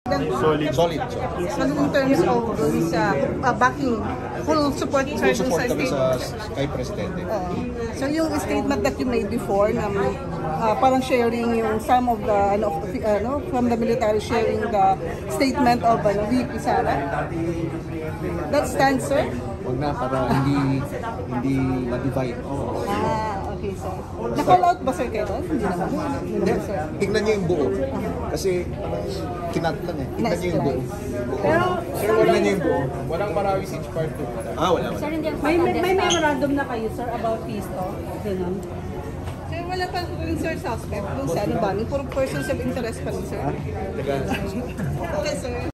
AFP chief on peace talks: 'Full support kami kay Presidente' | ABS-CBN News
Armed Forces of the Philippines (AFP) Chief of Staff General Romeo Brawner said on Wednesday, December 6 that the military is giving its "full support" to President Ferdinand Marcos Jr. following his recent grant of amnesty to former communist rebels.

Brawner made the statement after media reports quoted him as saying some soldiers shared the stance of Vice President Sara Duterte, who recently urged Marcos to review his amnesty proclamations and opposed an agreement to restart peace talks with communist rebels.

The AFP chief declined to elaborate on the reports, saying, "Huwag na, para hindi ma-divide."